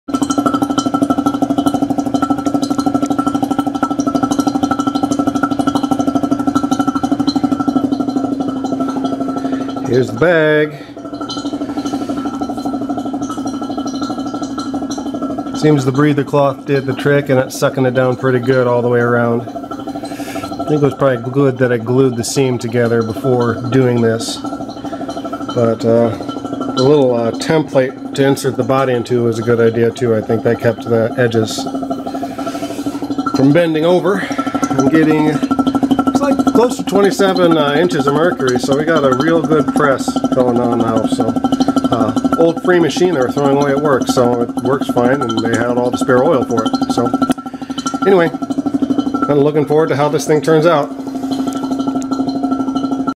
Vacuum bag pressing veneer on guitar top
My first guitar build needed a bit of sprucing up so I stripped it down and am gluing a really nice looking veneer on.